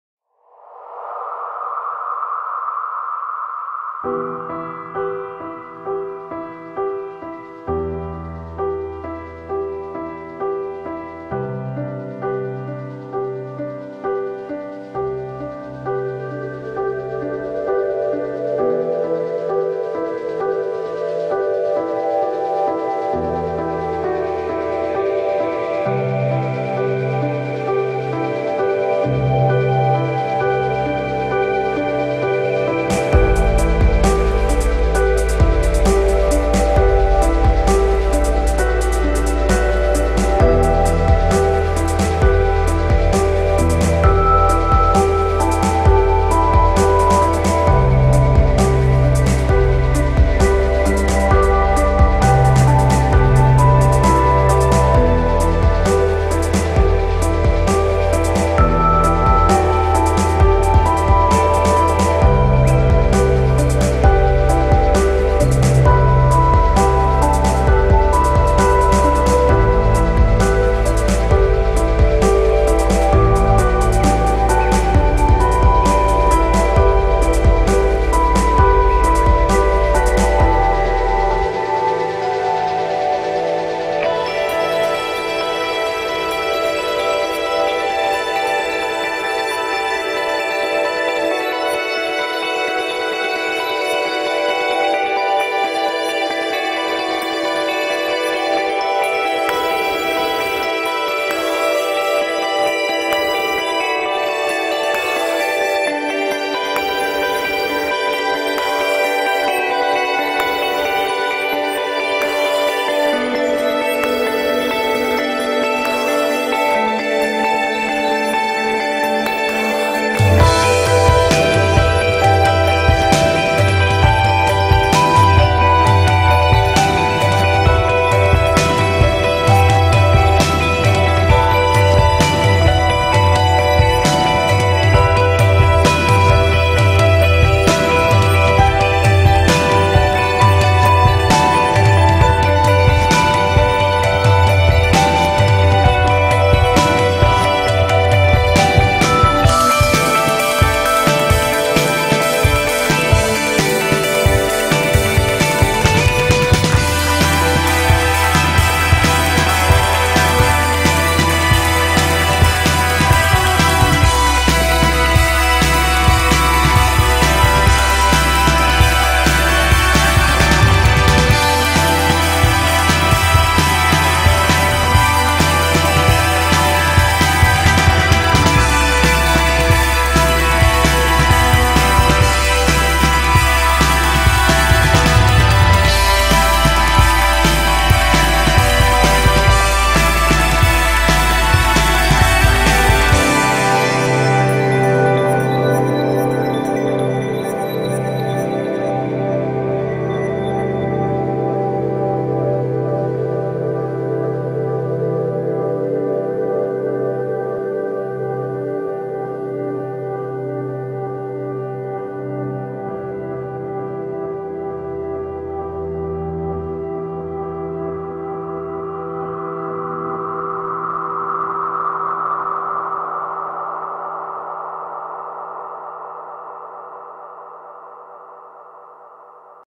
Timelapse New York, AfterGlow
timelapse  filmed this over one week in the winter. Scenes include Brooklyn Bridge, Central Park, Times Square, Grand Central, Wall Street, Met Museum, American Museum of Natural History, and the 9/11 Memorial.